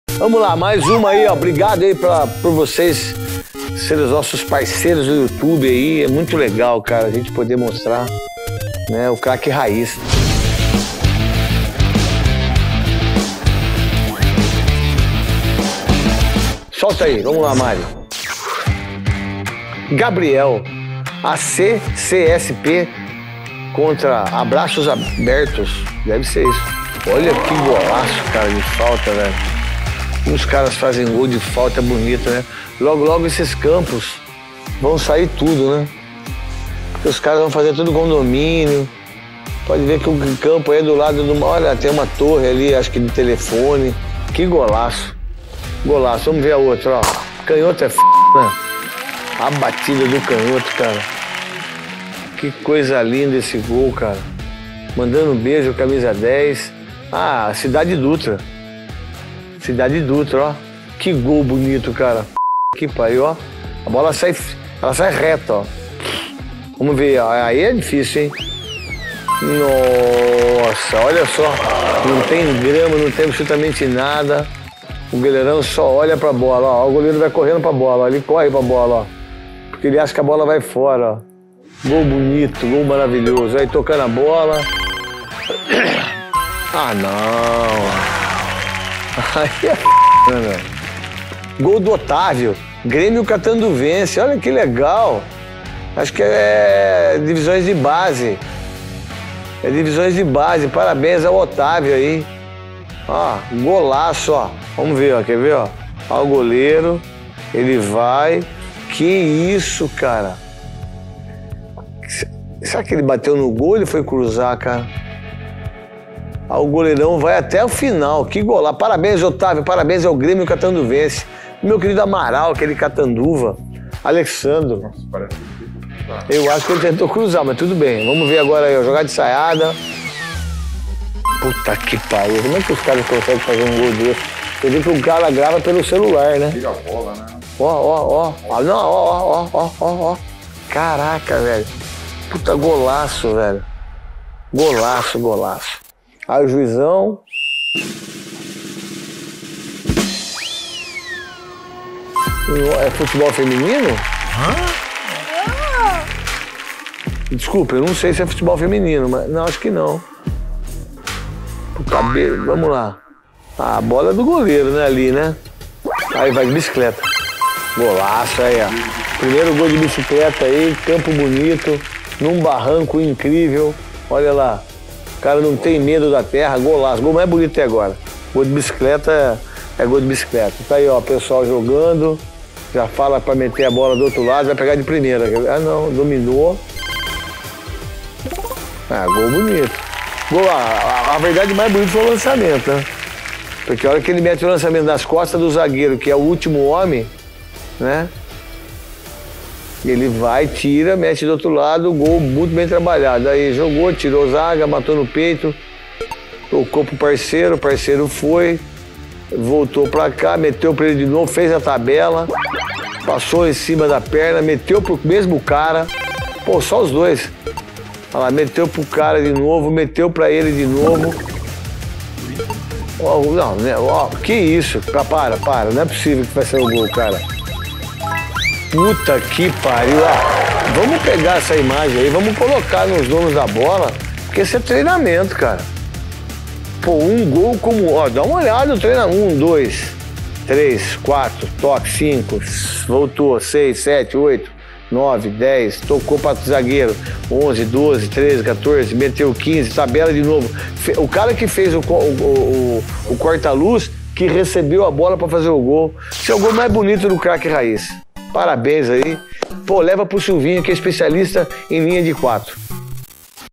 CRAQUE NETO VIBRA COM O GOL MAIS BONITO DA VÁRZEA | CRAQUE RAIZ #34
Se liga na programação:
Às terças rola o “Neto Responde”